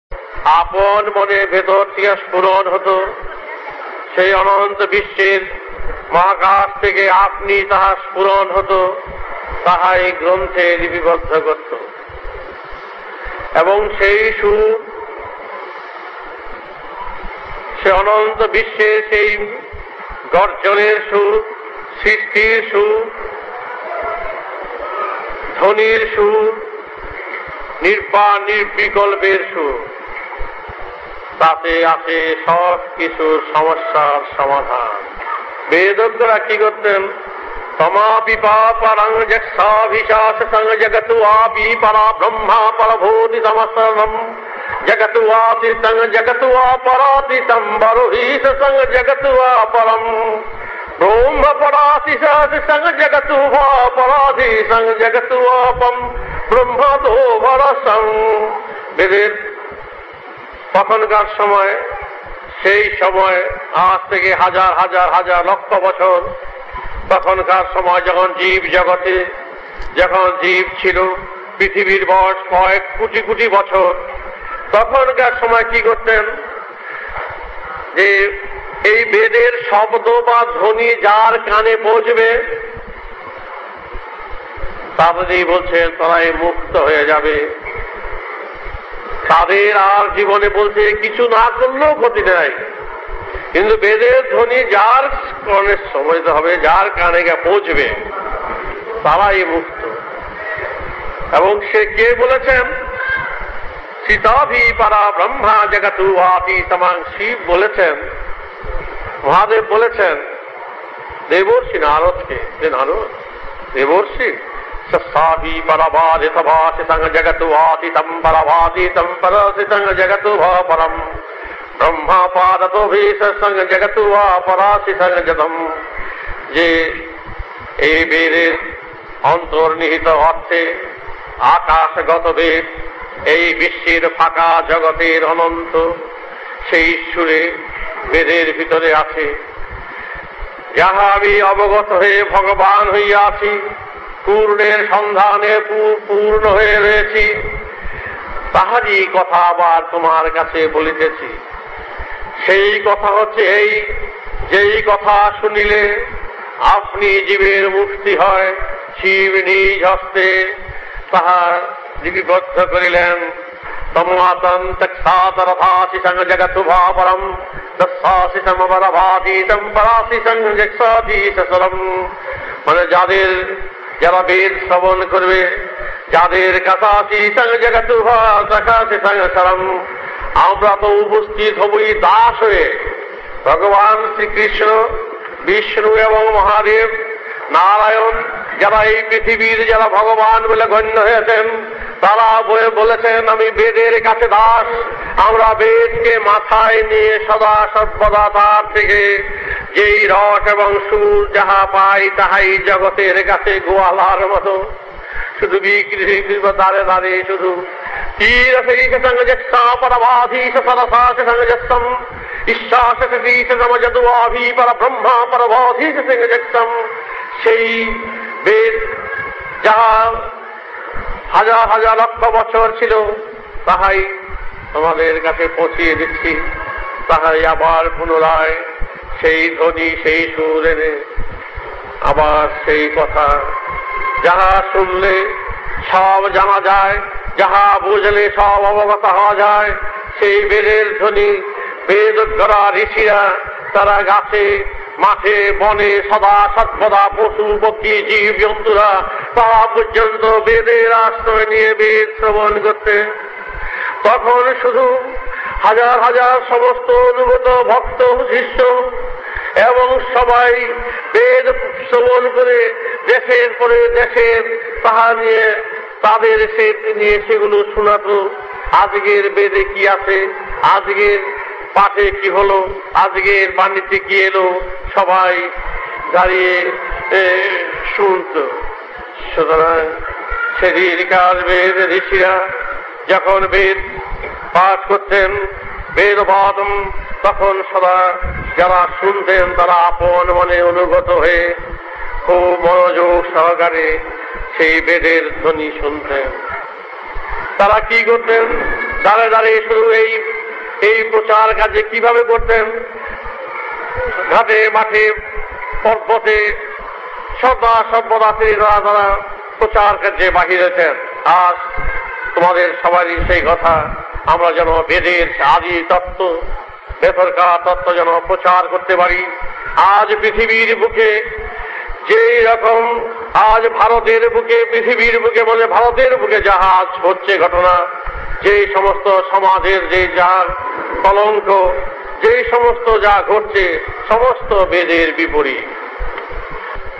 Sri Sri Thakur Balak Brahmachary: :Importance of Hearing The Mahavedadhyoni : মহাবেদোধ্বনির মহত্ব :
"মহাবেদোধ্বনির মহত্ব "
-: শ্রী শ্রী ঠাকুর বালক ব্রহ্মচারী মহারাজ:
:: পরমপিতার বেদতত্ত্বালোচনা::